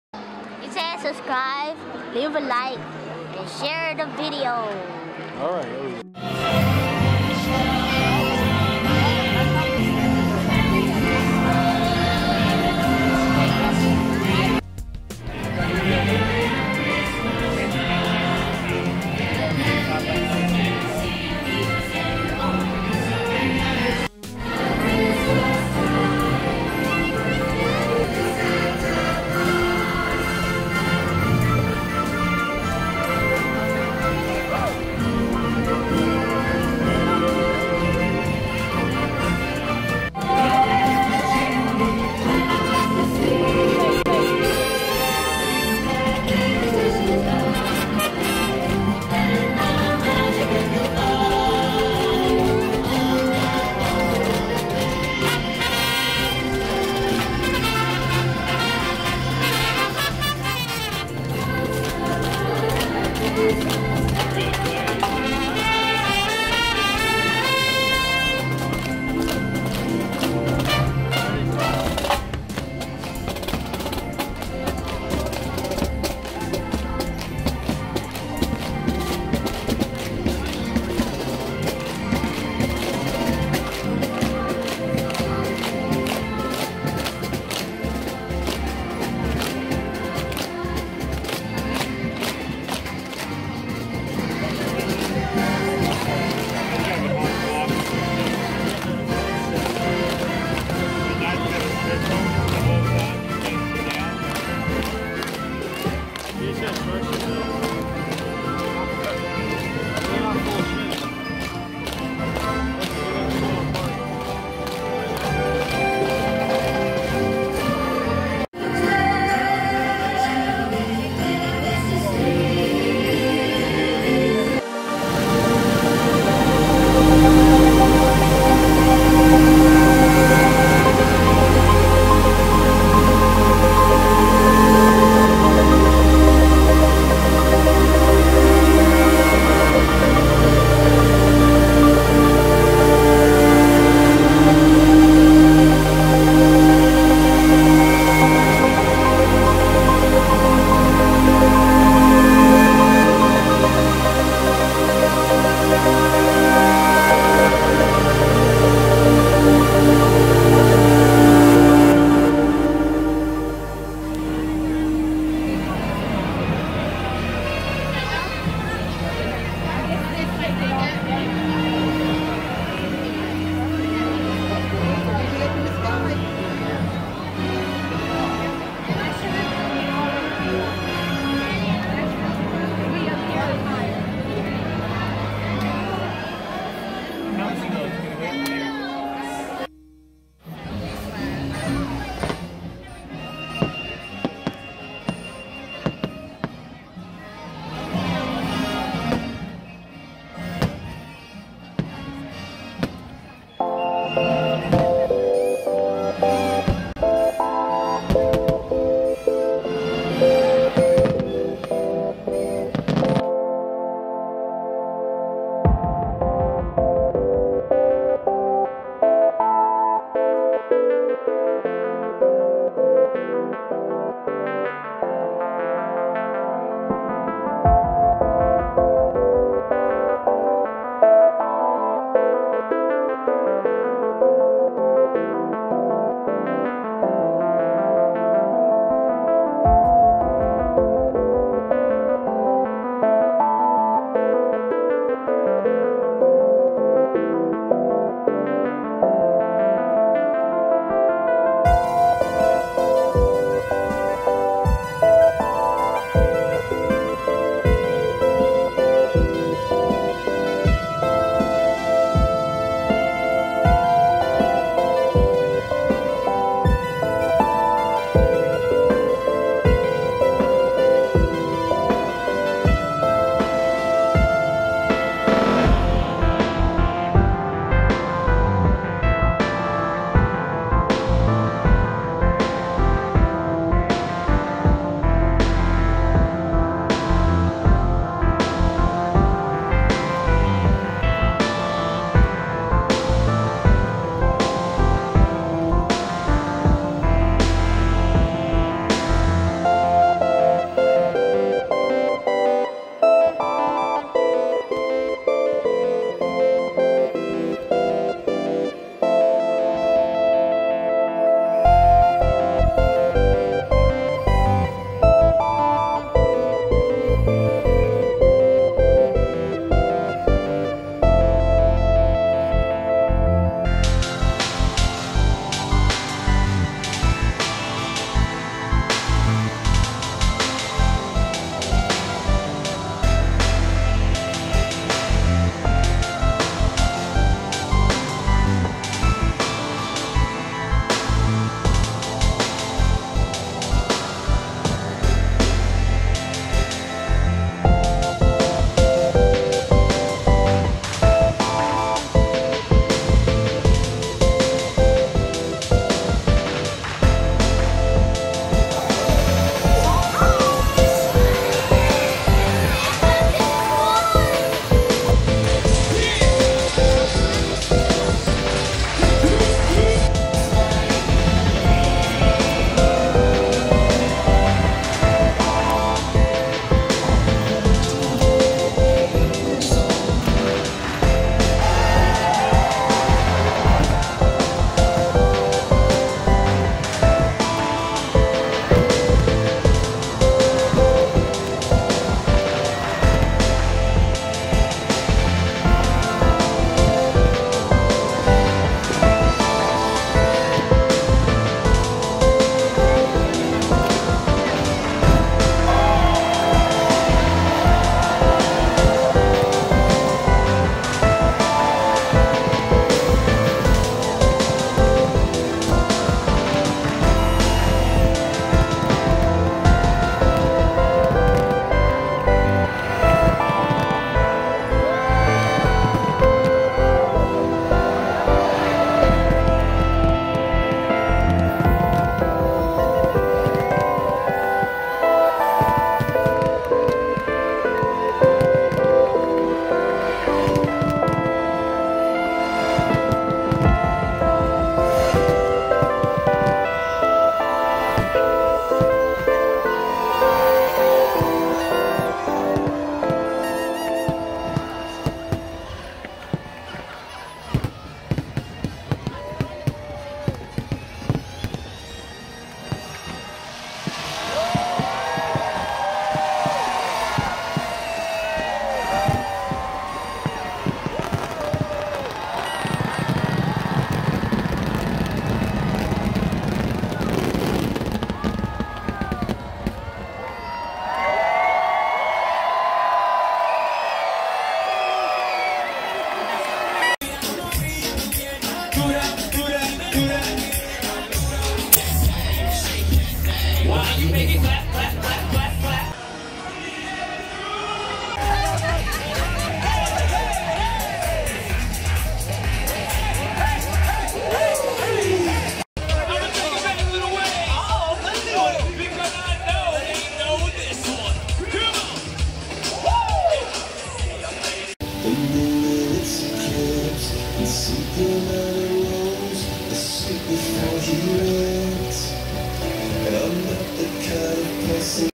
Magic Kingdom New Years Eve Walt Disney World Resort Orlando Florida
Walt Disney World Magic Kingdom New Years Eve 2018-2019 Part 3
Enjoyed bringing in the New Year at the most Magical Place on Earth. But now I'm home sick 😷😷😷😷😷😷